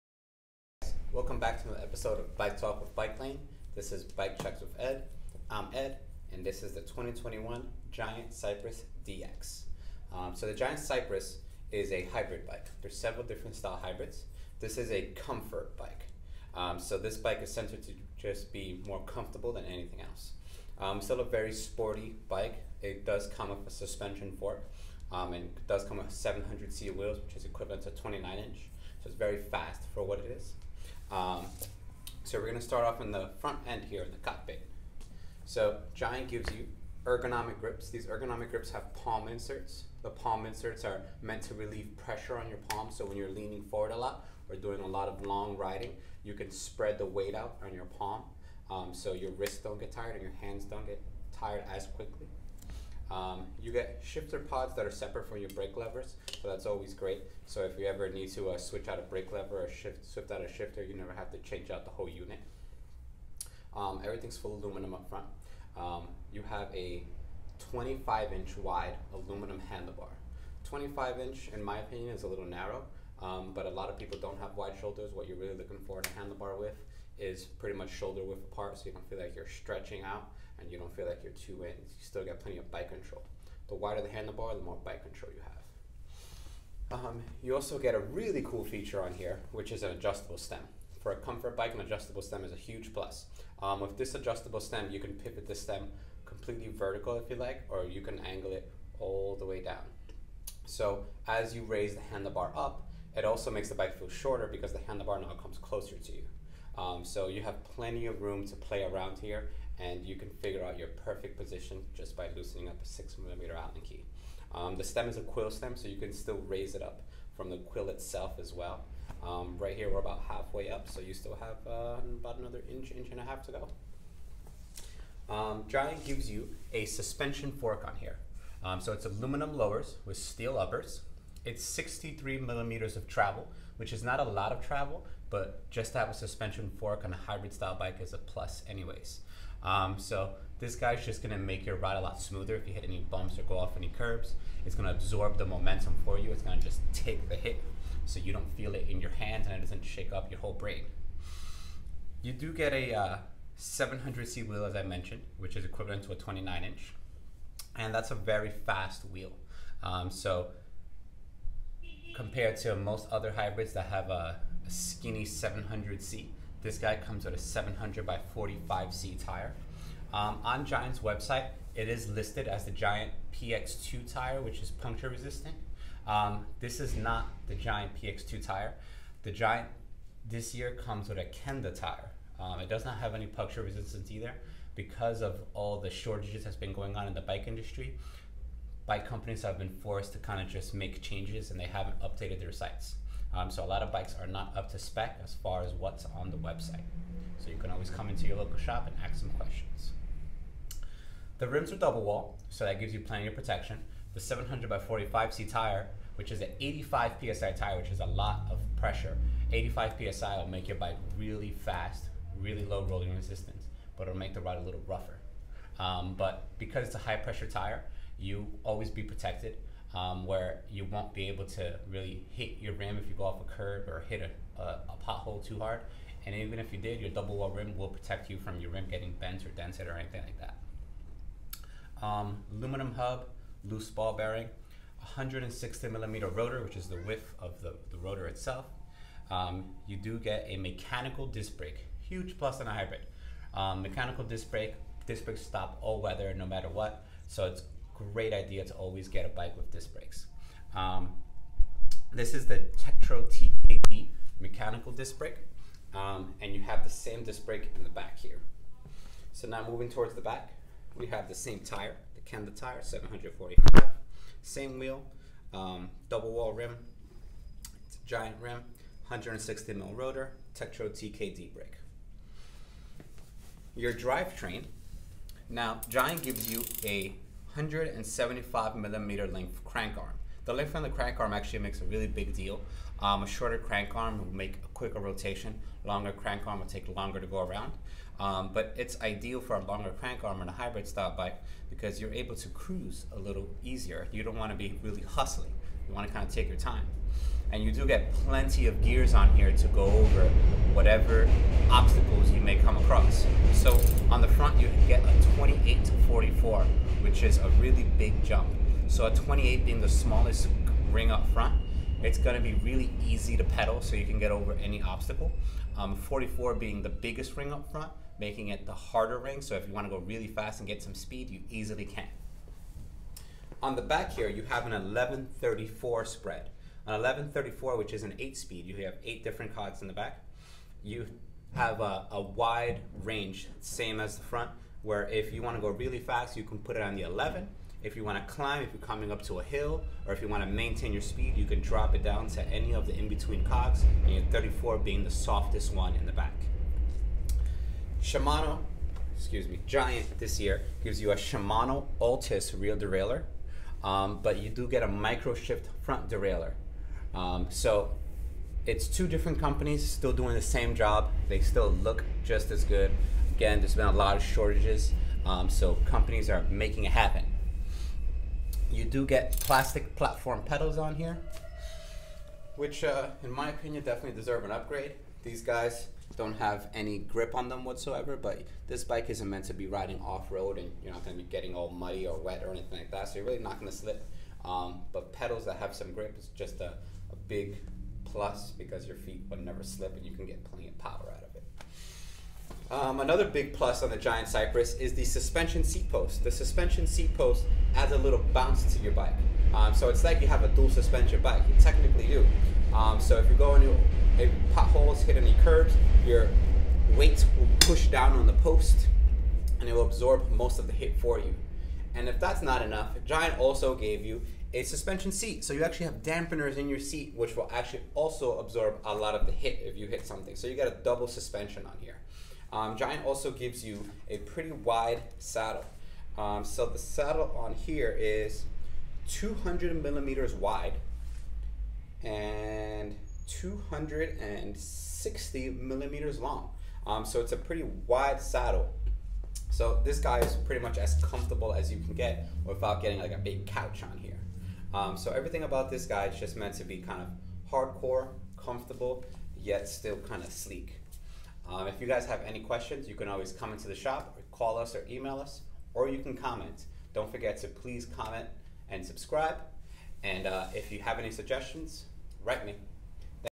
2021 Giant Cypress DX Bike check/review
A detailed review of the bicycles components and over all performance.